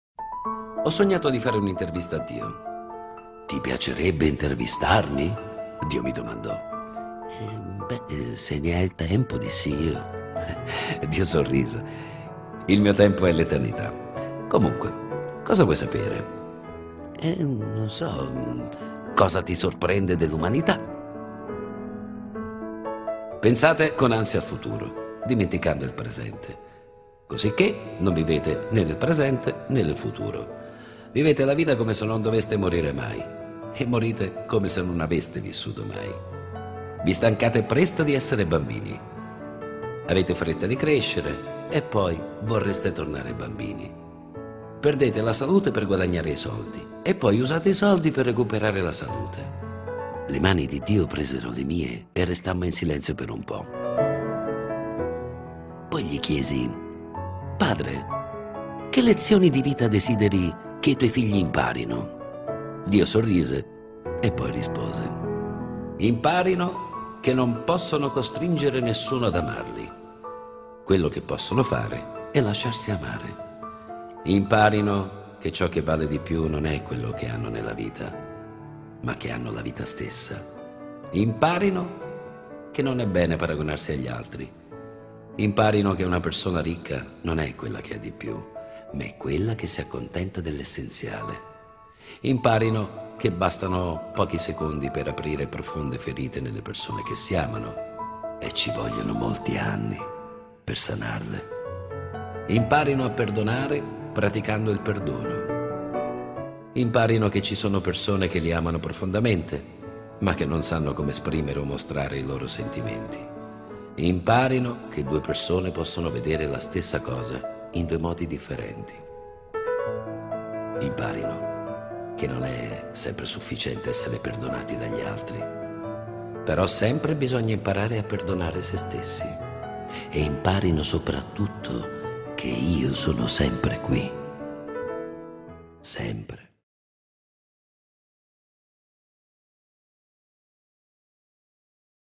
Intervista a Dio - Voce di Antonio Gerardi
Tratto da "Radio Kiss Kiss".

Ho sognato di fare un'intervista a Dio!

"Ti piacerebbe intervistarmi?",
Dio mi domandò.

"Se ne hai il tempo", dissi io.

Dio sorrise!

"Il mio tempo è l'eternità...
Che cosa vuoi sapere?"

"Che cosa ti sorprende dell'umanità?"

E Dio rispose...

"Pensate con ansia al futuro,
dimenticando il presente.

Così che non vivete
né nel presente, né nel futuro!

Vivete la vita come se non doveste morire mai,
e morite come se non aveste mai vissuto.

Vi stancate presto di essere bambini.

Avete fretta di crescere, e poi...
...vorreste tornare bambini!

Perdete la salute per guadagnare i soldi,
e poi usate i soldi per recuperare la salute!"

Le mani di Dio presero le mie
e restammo in silenzio per un pò.

Poi gli chiesi:

"Padre, che lezioni di vita desideri
che i tuoi figli imparino?"

Dio sorrise, poi rispose:

"Imparino che non possono costringere
nessuno ad amarli.
Quello che possono fare è lasciarsi amare!

Imparino che ciò che vale di più
non è quello che hanno nella vita,
ma che hanno la vita stessa!

Imparino che
non è bene paragonarsi agli altri!

Imparino che una persona ricca
non è quella che ha di più,
ma è quella che si accontenta dell'essenziale!

Imparino che bastano pochi secondi
per aprire profonde ferite
nelle persone che si amano,
e ci vogliono molti anni per sanarle!

Imparino a perdonare
praticando il perdono.

""Imparino che ci sono persone
che li amano profondamente,
ma che non sanno come esprimere
o mostrare i loro sentimenti.""

Imparino che due persone possono vedere
la stessa cosa in due modi differenti.

Imparino che non sempre è sufficiente
essere perdonati dagli altri...

...però sempre bisogna imparare
a perdonare se stessi.

E imparino che io sono sempre qui.
SEMPRE".